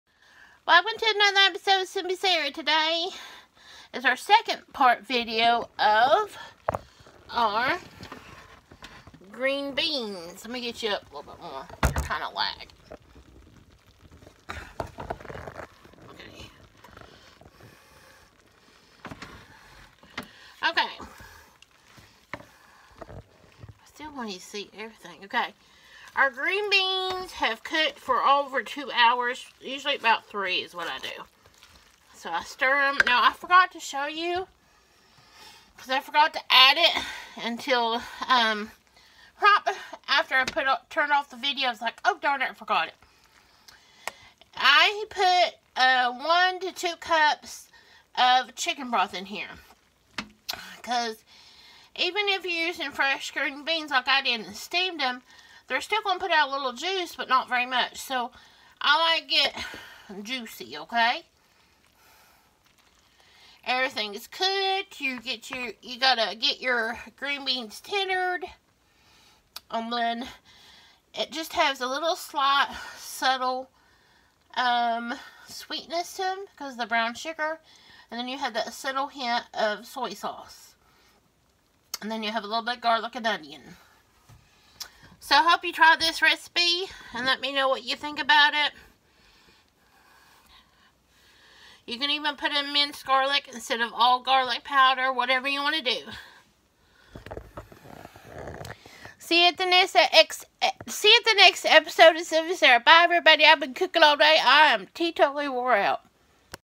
Crockpot Southern Smothered Green Beans Video 2
6 (14.5oz. cans) cut green beans drained(I used fresh garden green beans, and string them, rinse&drain, then steamed them till almost tender)
9 oz. package of real bacon bits
1 onion, chopped
1/2 Cup Brown Sugar
1 stick Melted Butter
4 tablespoons Tamari or Soy Sauce
1 tablespoons garlic powder
1 tablespoons onion powder
1 teaspoon of black pepper
1 1/2 cups of chicken broth(unsalted)
Put green beans and bacon bits, chopped onion in crockpot.
Mix melted butter, brown sugar, seasoning, soy sauce together, and pour over green beans and everything, and mix to combine.
Now pour over chicken broth and mix again.
Put lid on crockpot ,cook on high for 2 to 4 hours.

FOR OVEN WAY:
Bake 1 hour in 350º F oven.

Mailing Address:
Sara Potter
Louisa, Kentucky  41230